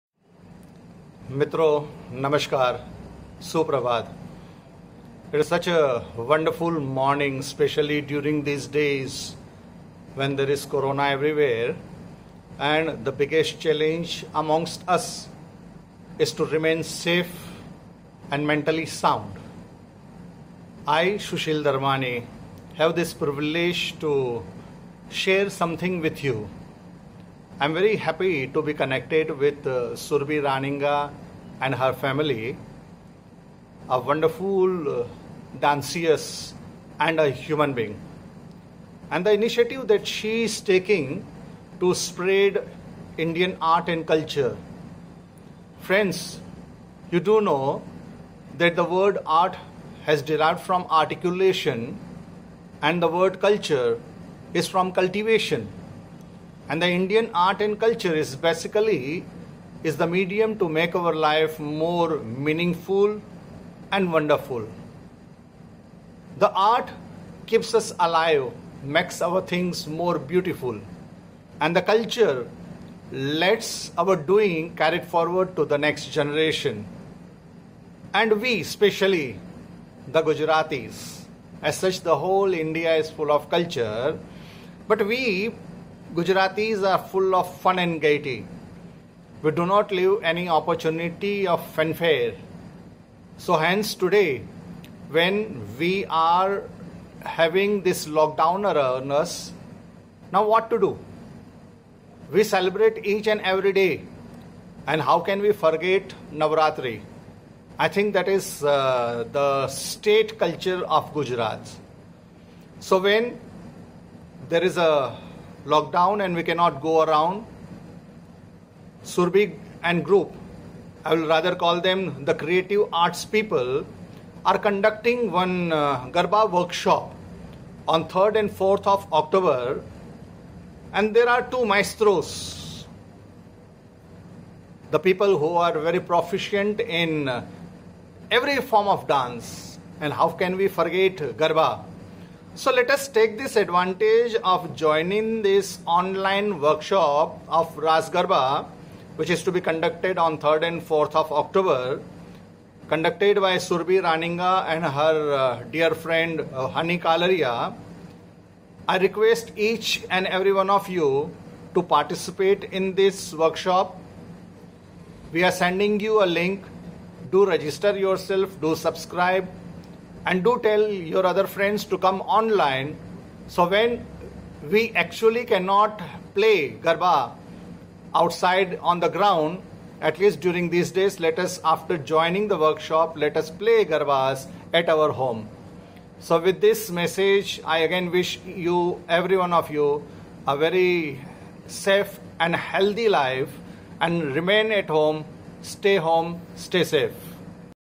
ONLINE GARBA TRAINING in aid of AKSHAYA PATRA - DR SUSHIL DHARMANI
A GLOBAL ONLINE GHARBA DANCE WORKSHOP IN AID OF AKSHAYA PATRA. Join Award Winning Dance Maestro Honey Kalaria & Indian folk dance expert, Surbhi Raninga for a two day online workshop to learn stunning modern, traditional and ancient Gharba Dance steps. There is also an option to order a step by step 12 module garba course online, with 20% of proceeds being donated to charity if booked before deadline date.